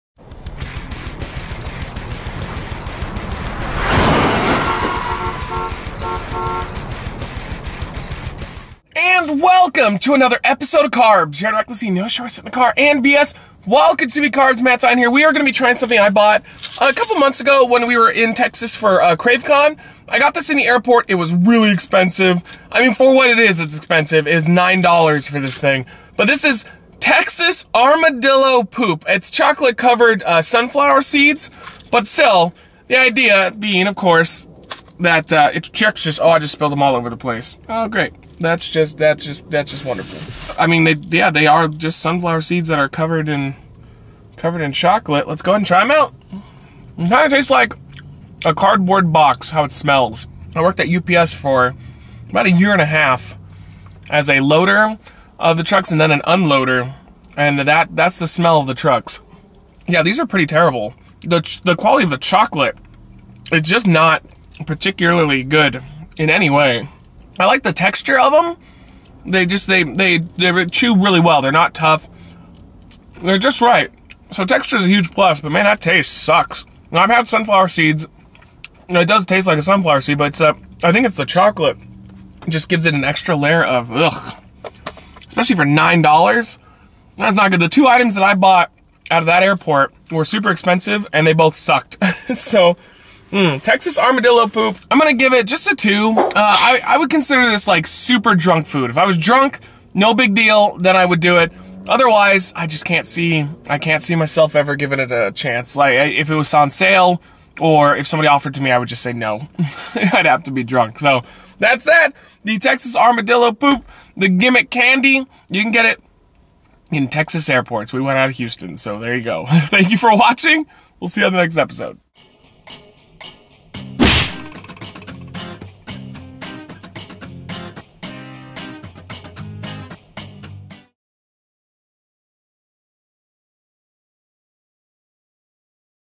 Texas Armadillo Poop Review - CarBS
Welcome to CarBS! On this show Matt Zion will be reviewing food/drinks in his car to get them out to the fans asap! Today Zion reviews Texas Armadillo Poop!